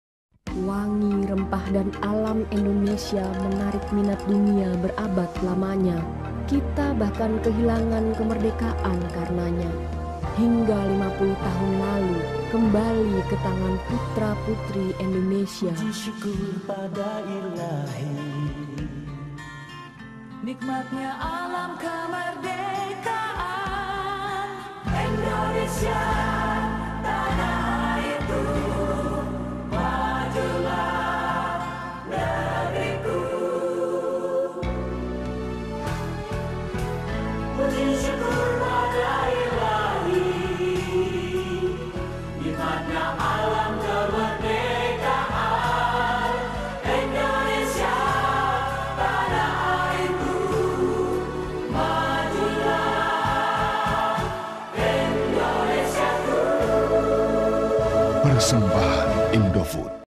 Iklan Indomie - 50 Tahun Kemerdekaan RI (1995) @ Indosiar, TPI, RCTI, SCTV, & ANteve
Iklan ini pernah ditayangkan di Indosiar, TPI, RCTI, SCTV, & ANteve pada perayaan HUT Kemerdekaan RI Ke-50 tahun 1995.